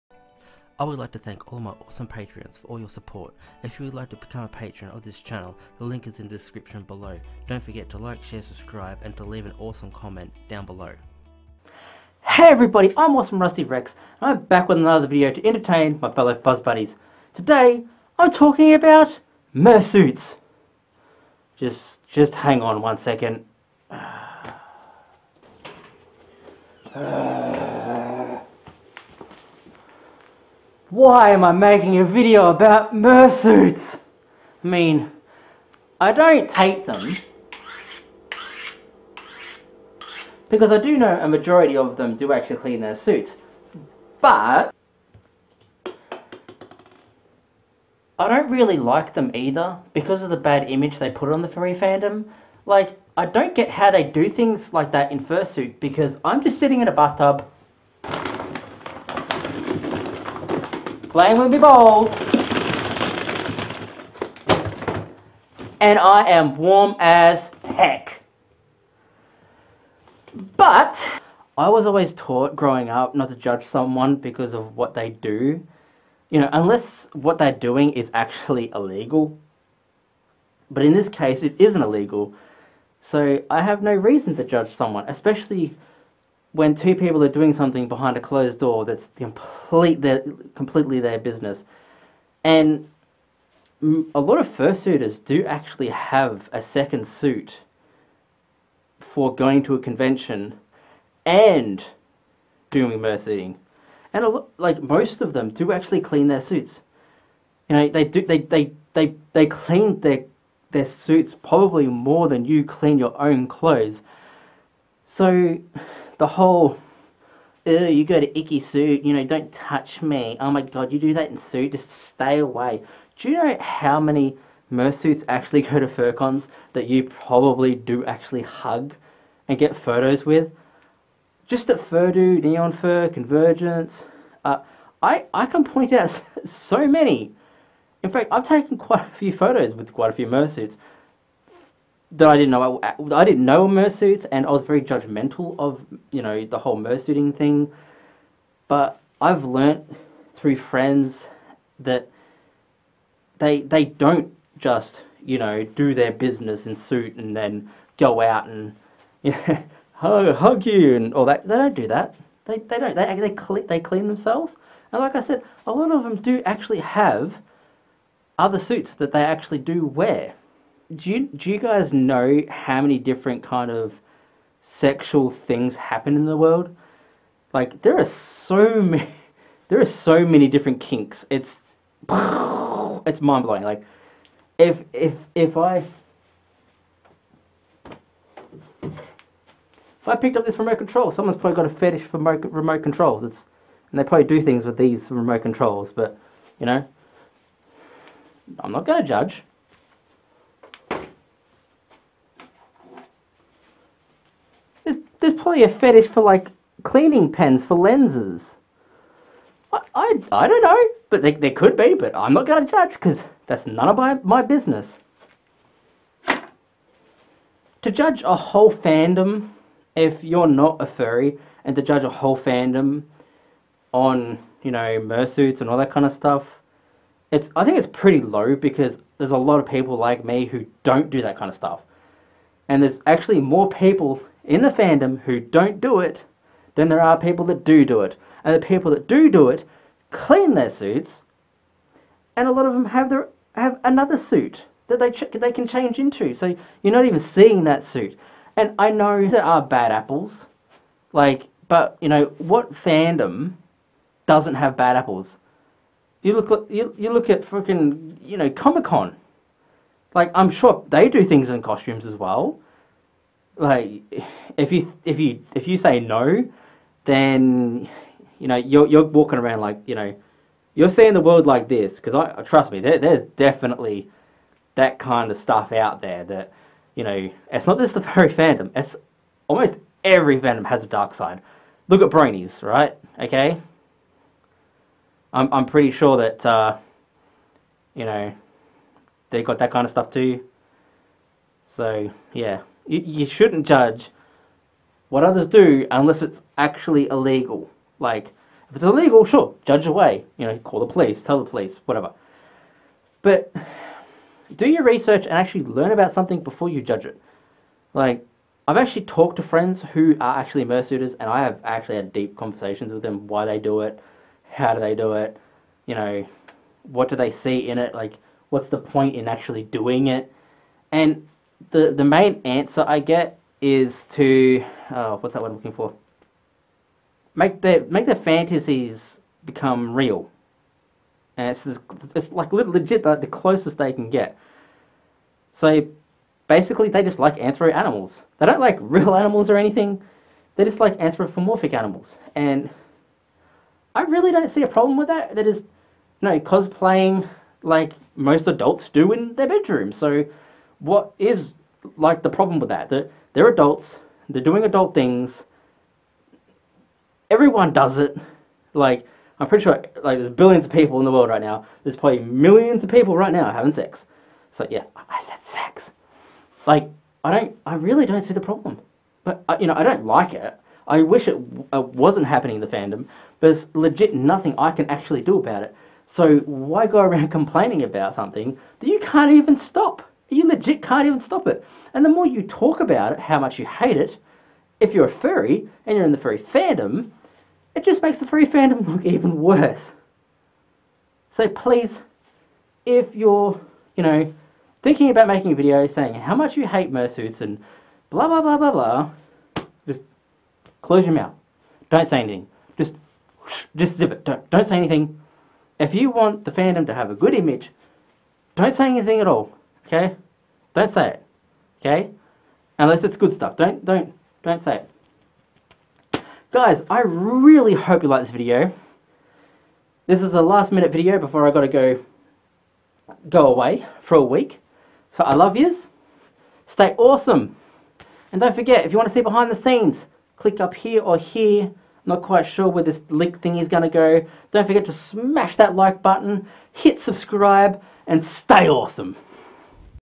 Why i Hate Murrsuits Or do i ?
So when i joined the fandom back in 2015 i really didn't know anything about the more adult side of the fandom but i have made so many new friends and you know some friends cant shut up and  like to talk about what they get up to and what they like to do behind closed doors even if you don't want to know so i thought i would have my say and let people know exactly how i feel about the darker side of things